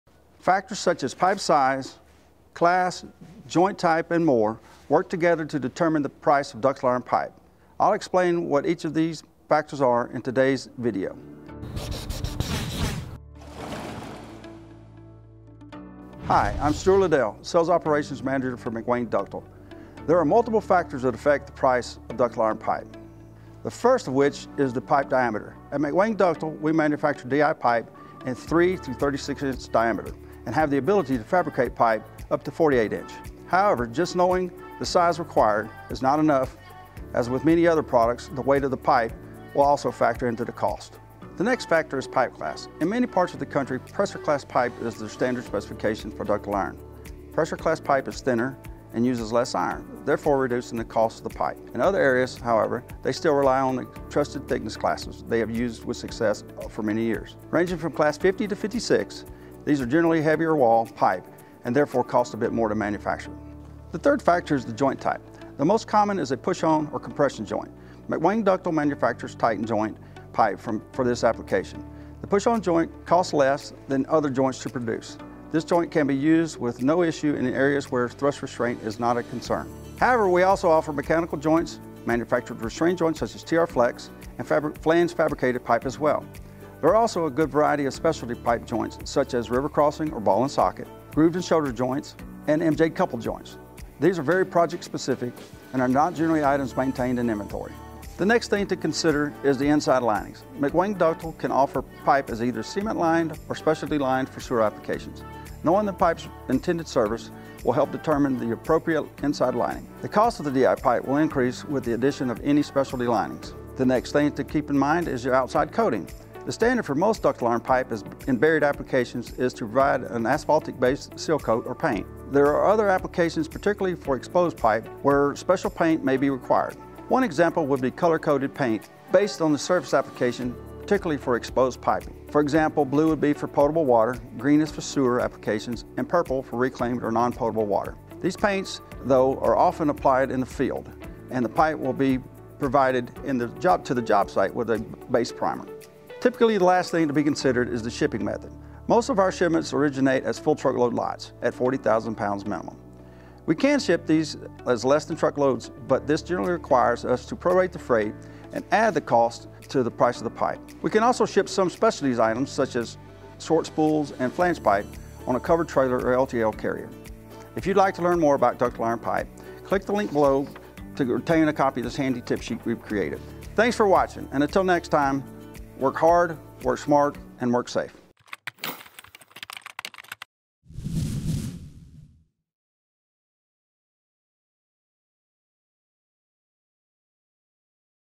How Much Does Ductile Iron Pipe Cost?
Factors such as pipe size, joint type, pipe class, inside and outside linings, and shipping costs all affect the price of ductile iron pipe. In this video Stuart Liddell breaks them down one by one.

If you would like to learn more about the cost of ductile iron you can read Stuart's article

MDV000065